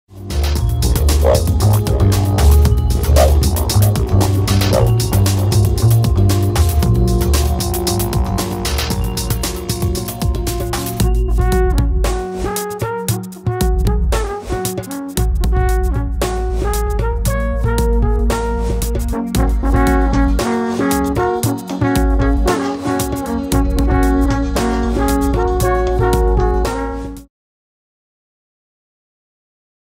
alperidoo teaser
Alphorn on wheels through Paris, full video coming soon!